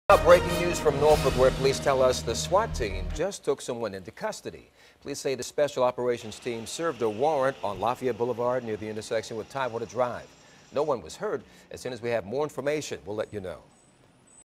Warrant served on Lafayette Boulevard
Police say one person was taken into custody after a warrant was served on Lafayette Boulevard, Monday morning.